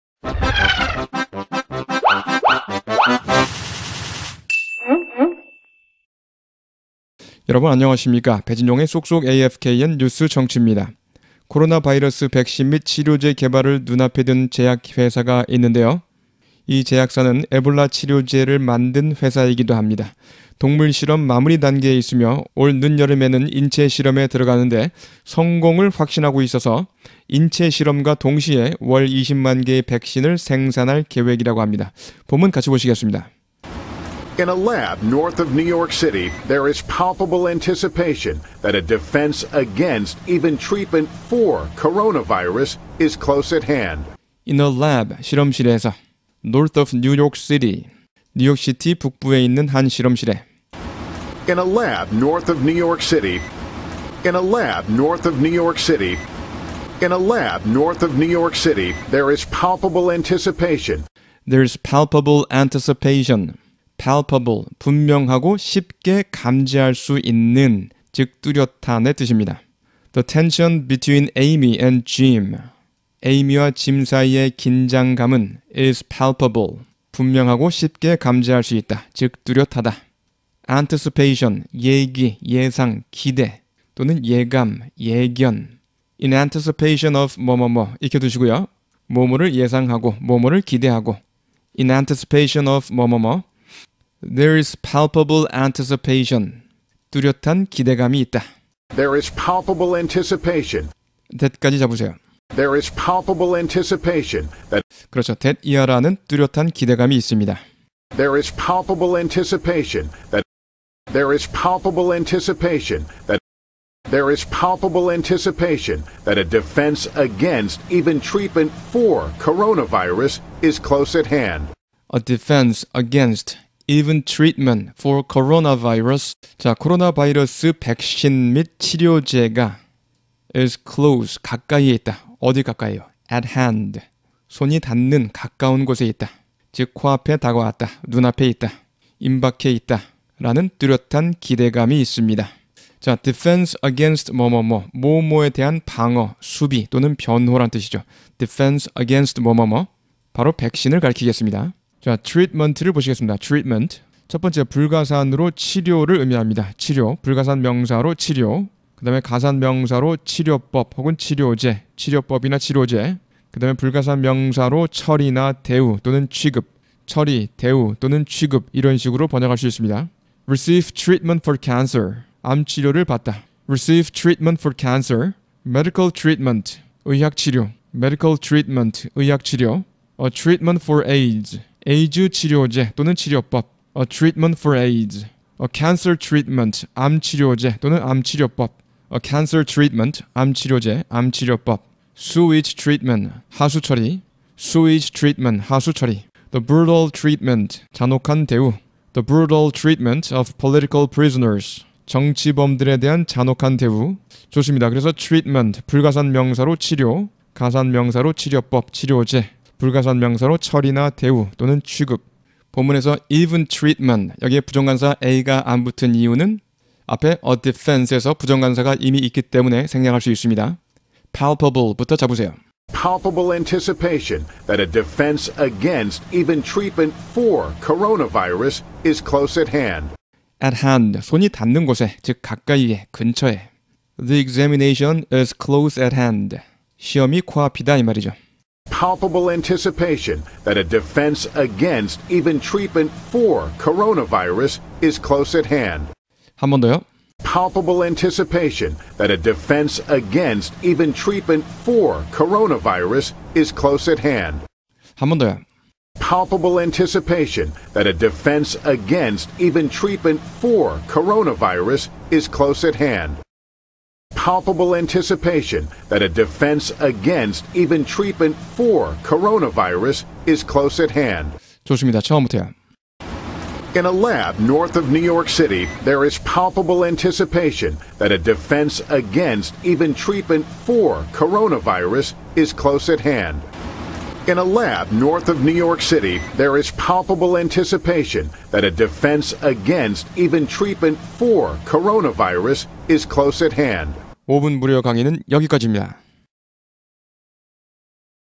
코로나바이러스 백신
코로나바이러스 백신 및 치료제 개발을 눈앞에 둔 제약회사가
있는데요. 이 제약사는 에볼라 치료제를 만든 회사이기도 합니다.
동물실험 마무리단계에 있으며 올 늦여름에는 인체실험에 들어가는데
성공을 확신하고 있어서 인체실험과 동시에 월 20만개의 백신을
생산할 계획이라고 합니다.